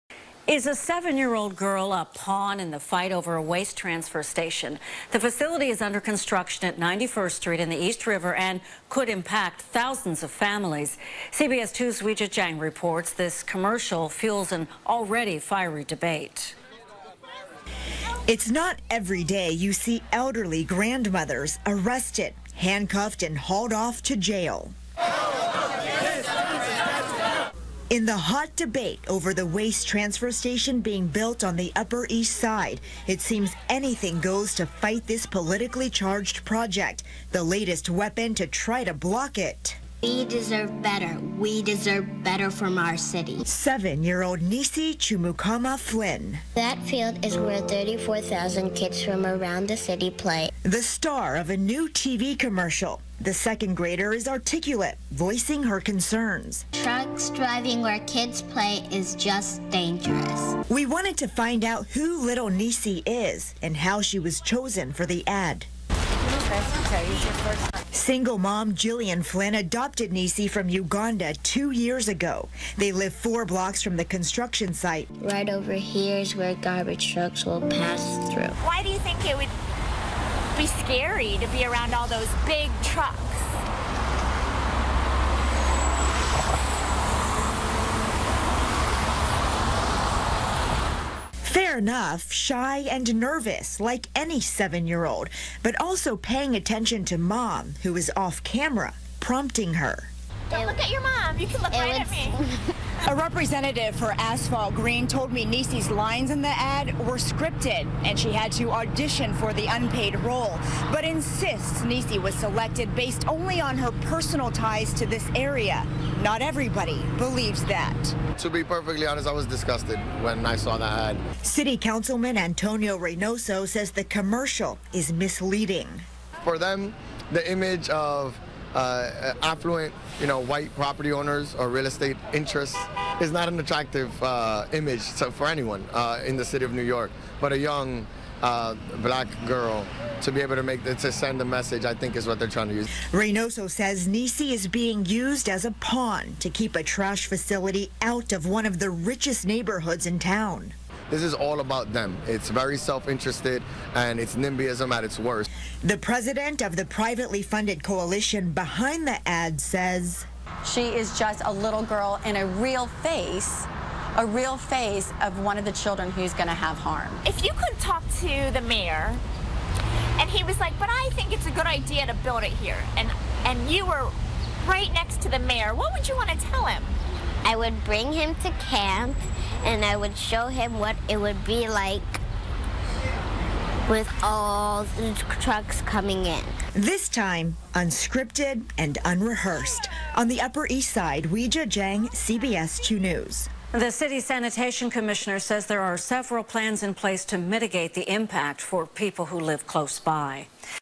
New Commercial Fuels Debate Over Upper East Side Transfer Station
As CBS 2's Weijia Jiang reported, some say a young spokesperson is being used as a political pawn.